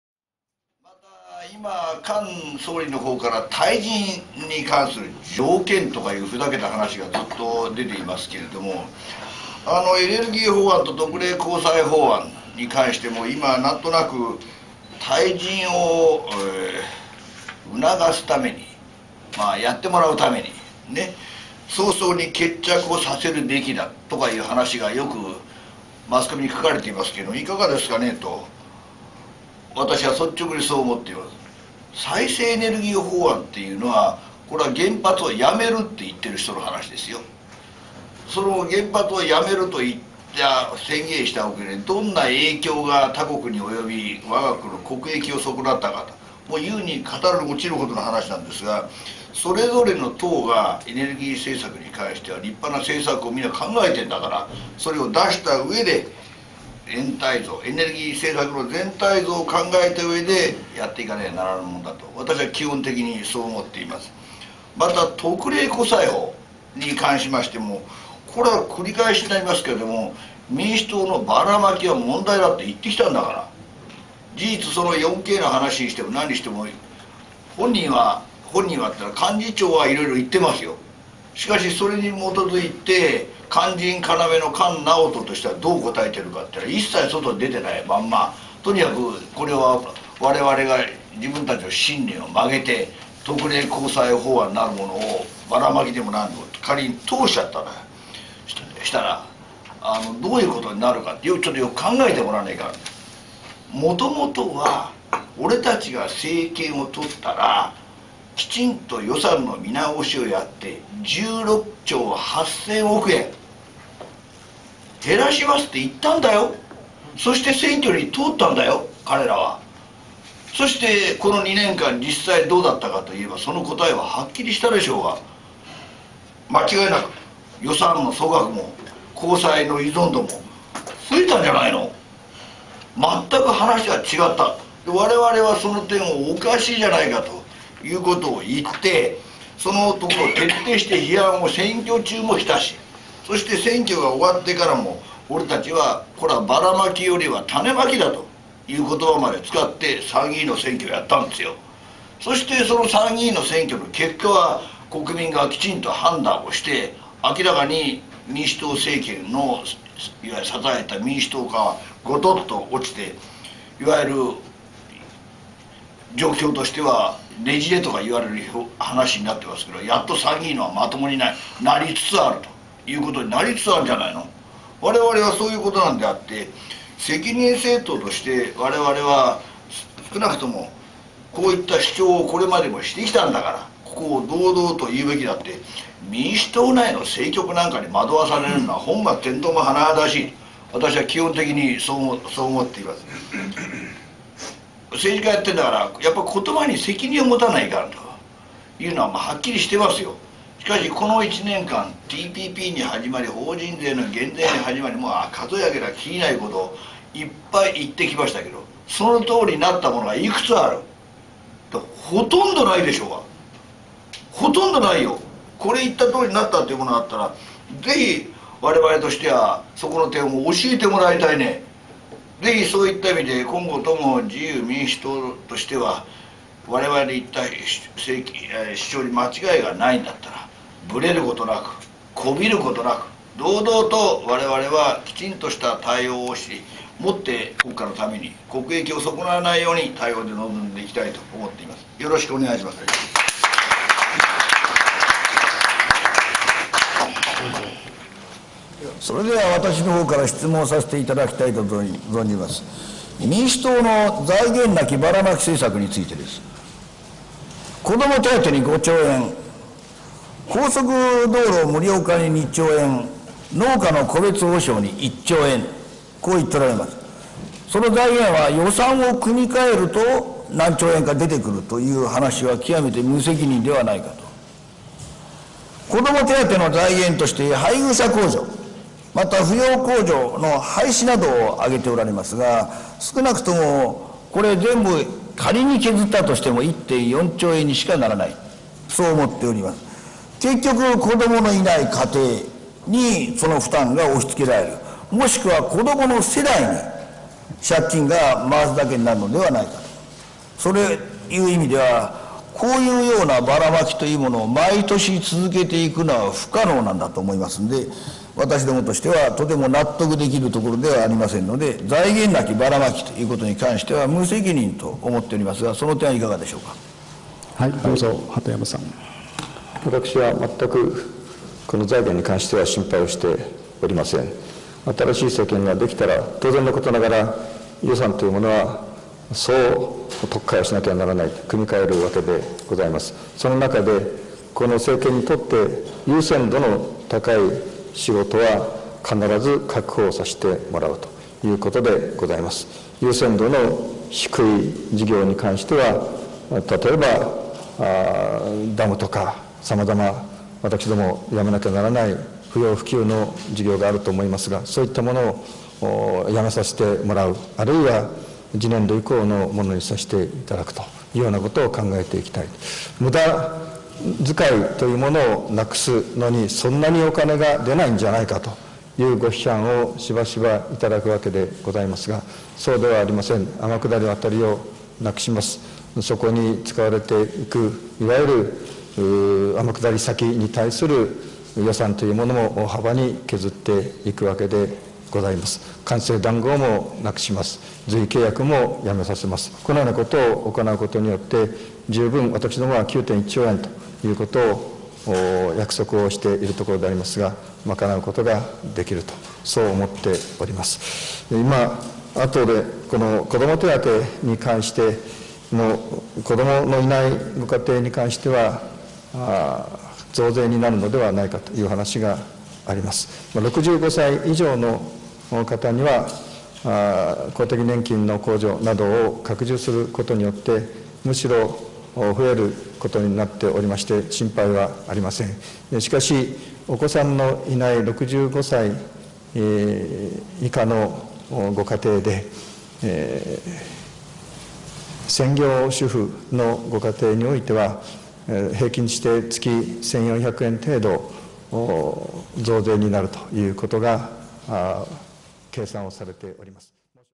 麻生太郎 為公会例会～2009年党首討論
麻生・元総理大臣は「再生可能エネルギー買い取り法案と赤字国債発行法案を菅総理大臣の退陣を促すため、早々に決着させるべきだという話があるが、いかがなものかと思う。自分たちの信念を曲げて赤字国債発行法案を通したらどういうことになるか、よく考える必要がある。民主党内の政局に惑わされて対応を変えるのは本末転倒だ」と述べました。